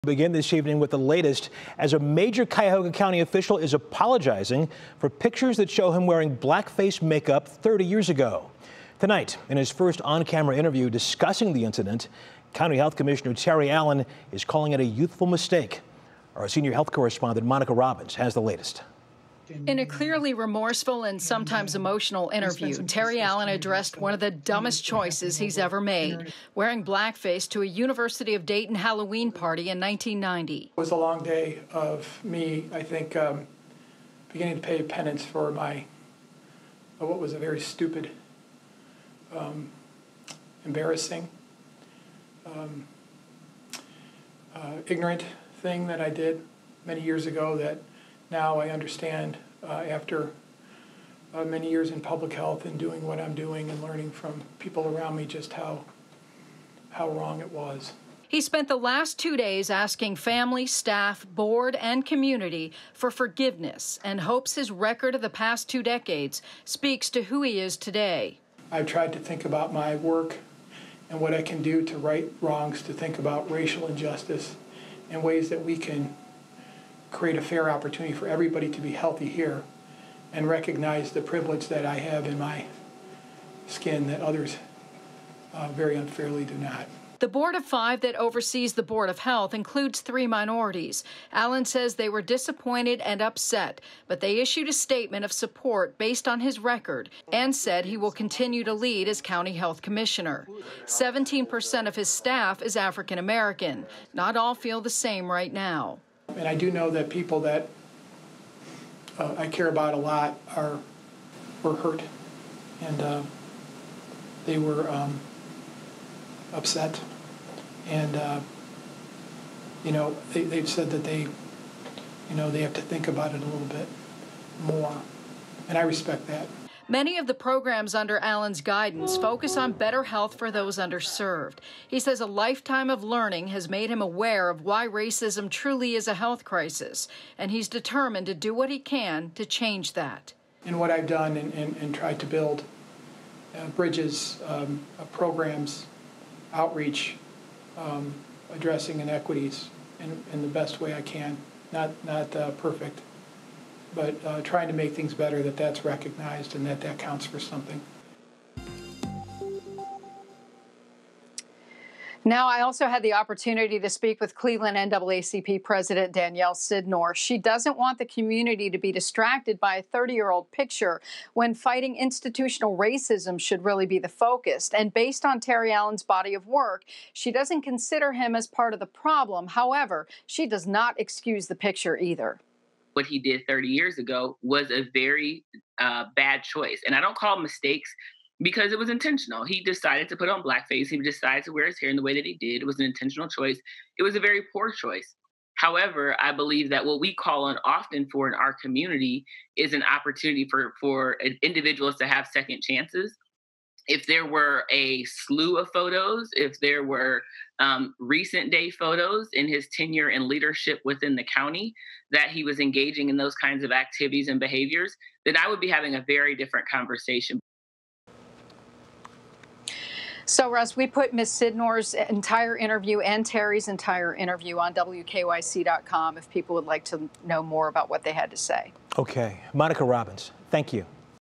Cuyahoga County Board of Health commissioner Terry Allan apologizes for blackface photo in interview
"People make mistakes. My hope is this doesn't define me and the work that I've done since that time." Allan sat down to apologize for the for the incident.